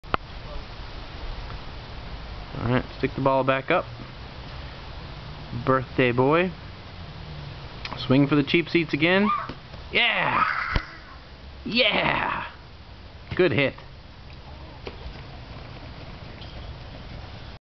Kerek and some Tee Ball
Kerek tries out some tee ball on his second birthday